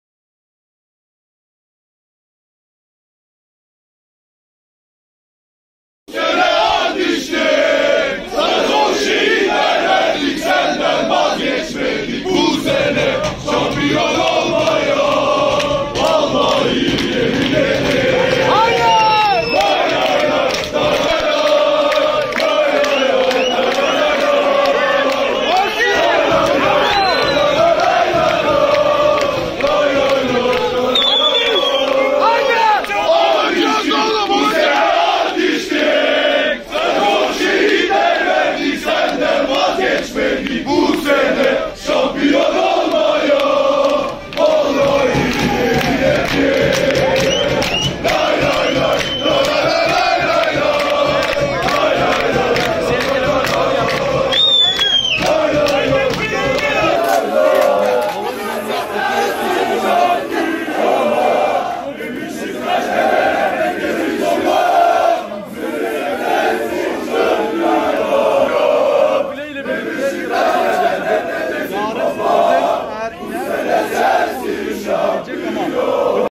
Bordo mavili taraftarlar, Trabzonspor kafilesini havalimanında karşıladı
LiderTrabzonspor I TS I Haberleri
GÖRÜNTÜ DÖKÜMÜ :
- Trabzonspor kafilesini havalimanında karşılayan taraftarlardan görüntü
- Kafilenin otobüse binmesi ve taraftarların tezahüratları
TRABZON (AA) - Spor Toto Süper Lig'in 16. haftasında deplasmanda karşılaştığı Fraport TAV Antalyaspor'a 2-1 mağlup olan Trabzonspor, havalimanında bordo-mavili taraftarlarca karşılandı.
Antalya Stadı'nda oynanan müsabakadan mağlup ayrılan ve Spor Toto Süper Lig'de 27 maçlık yenilmezlik serisi son bulan bordo-mavililer, maçın ardından uçakla Trabzon'a döndü. Bordolu-mavili takımı, Trabzon Havalimanı'nda bir grup taraftar karşıladı.
Meşaleler yakarak, çeşitli tezahüratlarla futbolculara sevgi gösterisinde bulunan taraftarlar, takımlarına moral verdi. Taraftarların desteği altında takım otobüsüne binen bordo-mavili futbolcular, daha sonra havalimanından ayrıldı.